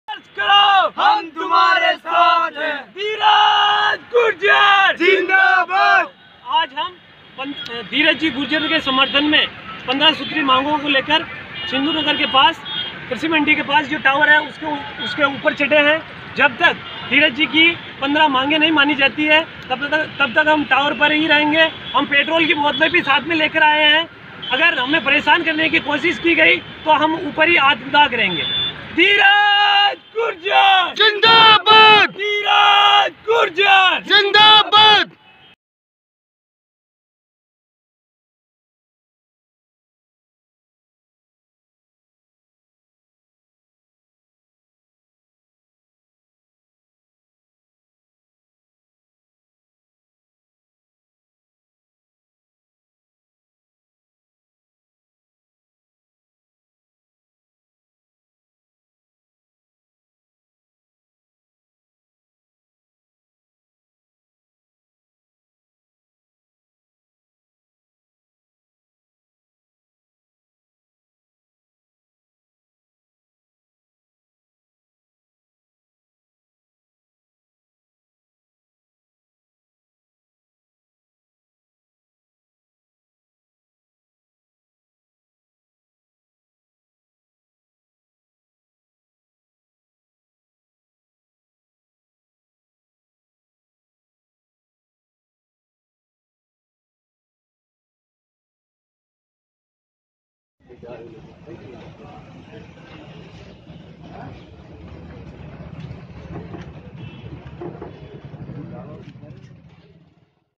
7 students on mobile tower मोबाइल टॉवर पर क्यो चढ़े 7 छात्र
भीलवाड़ा में 7 दिनों से अनशन पर बैठे विधायक धीरज गुर्जर के समर्थन में 7 छात्र मोबाइल टॉवर पर चढ़ गए है, पुलिस समझाइश के प्रयास कर रही है।